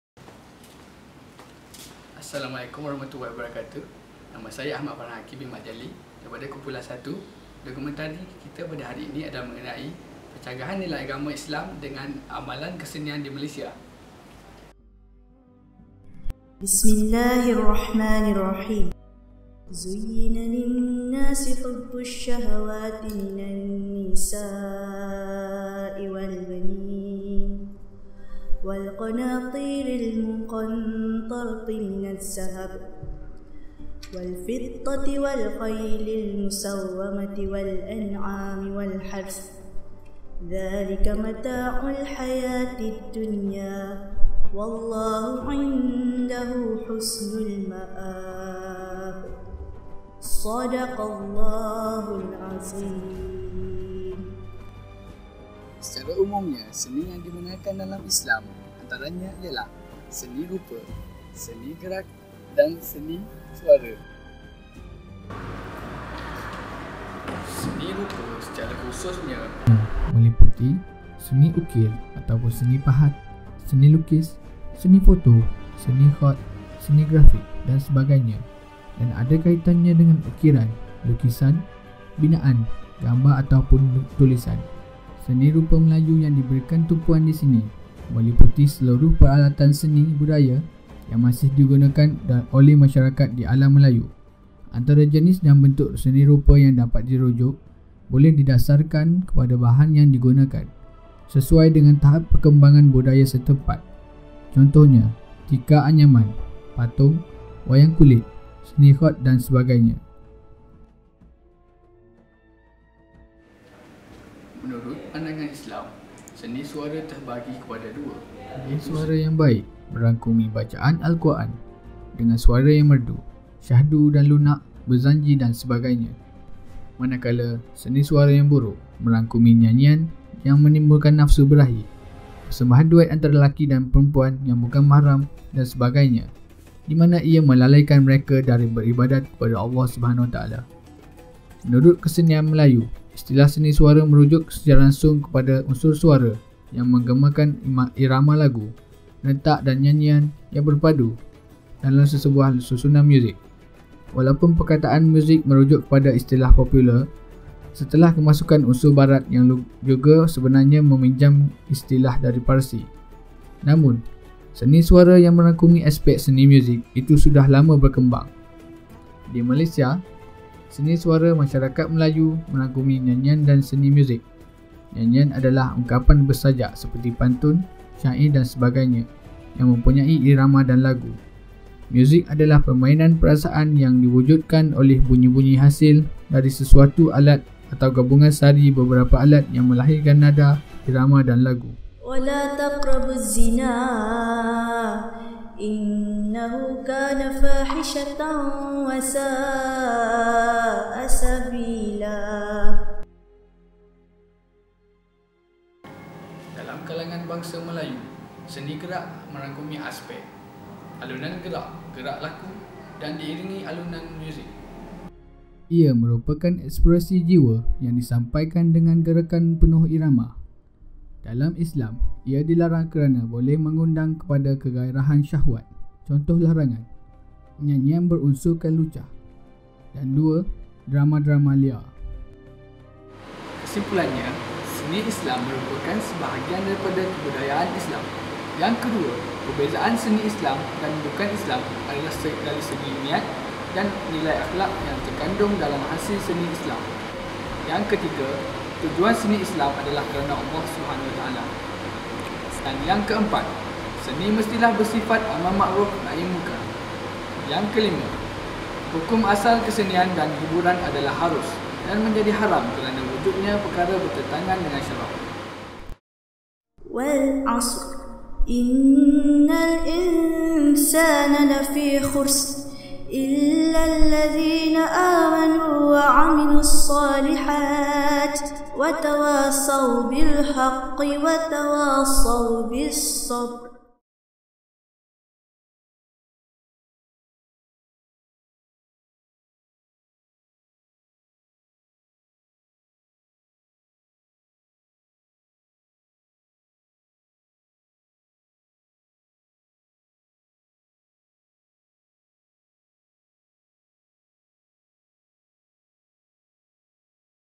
ASSIGNMENT001: Pencanggahan Seni dalam agama Islam
Video ini dibuat untuk tujuan Ilmiah serta untuk subjek Tamadun Islam dan Tamadun Asia Tenggara Pusat Pengajian Seni dan Matematik Sidang 2015/2016 Universiti Sains Malaysia.